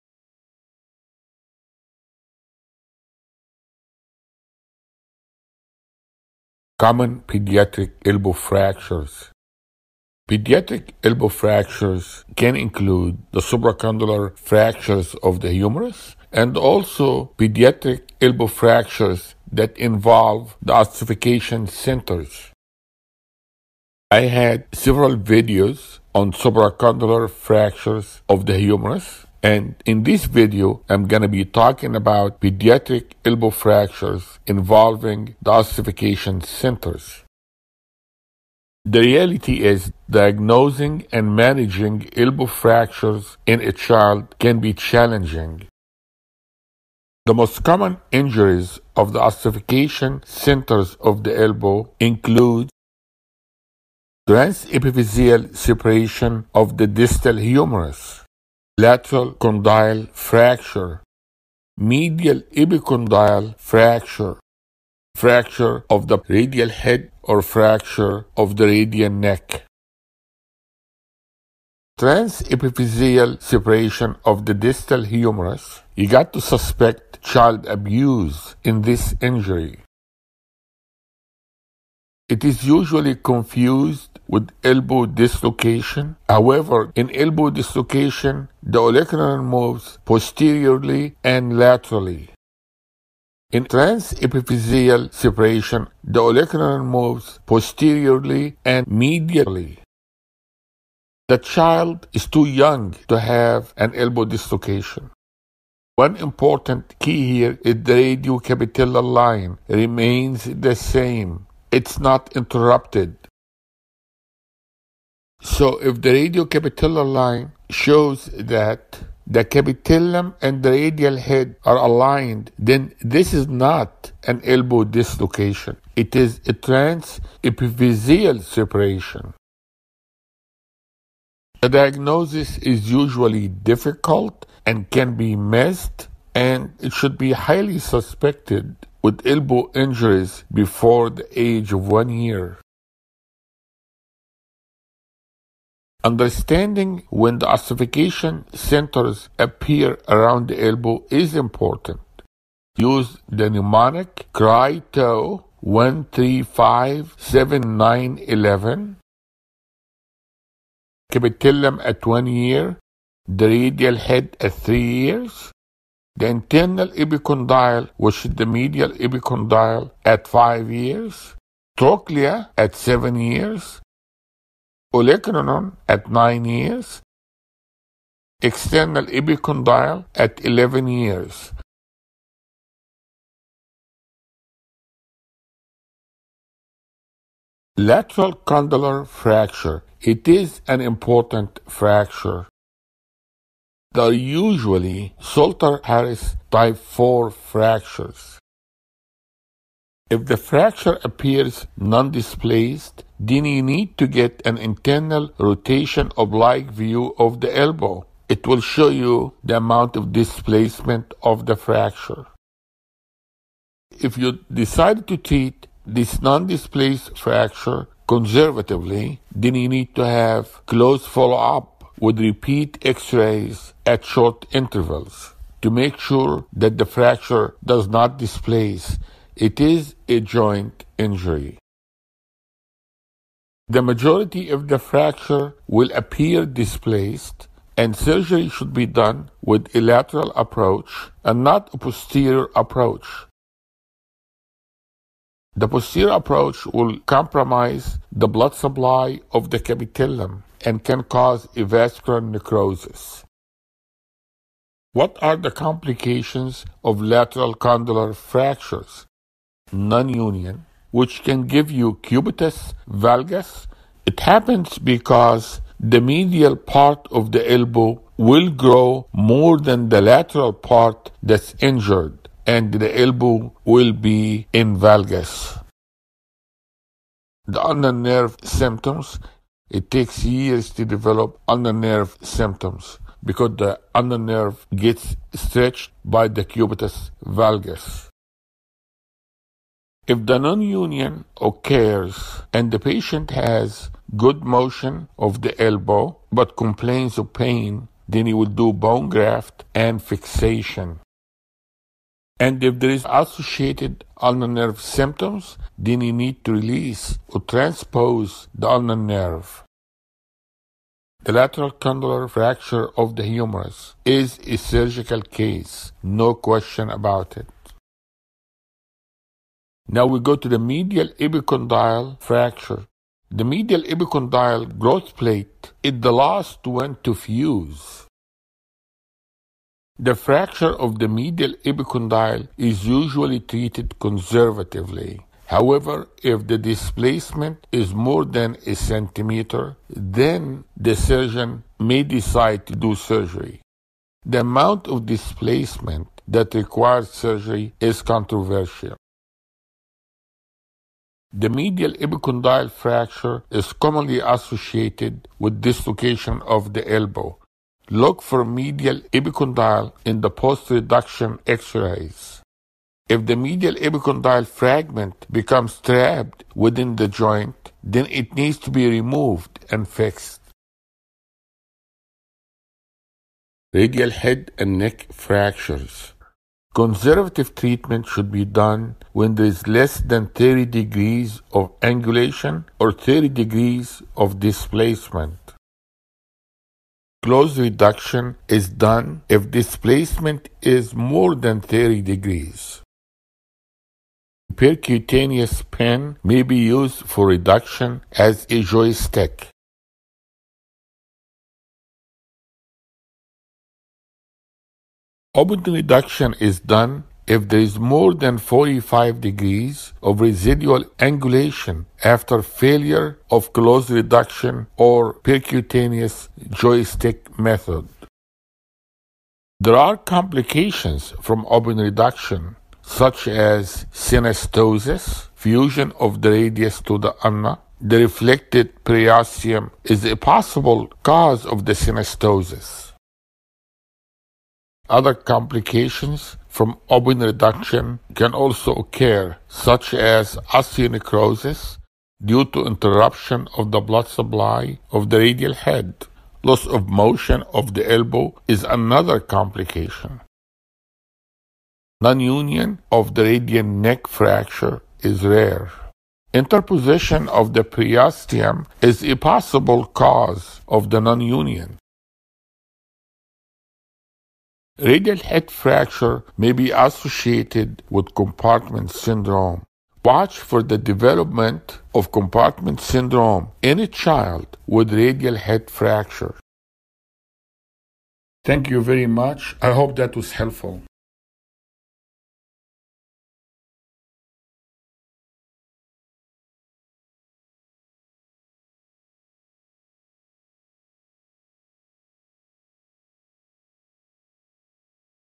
Common Pediatric Elbow Fractures - Everything You Need To Know - Dr. Nabil Ebraheim
Dr. Ebraheim’s educational animated video describes common pediatric elbow fractures.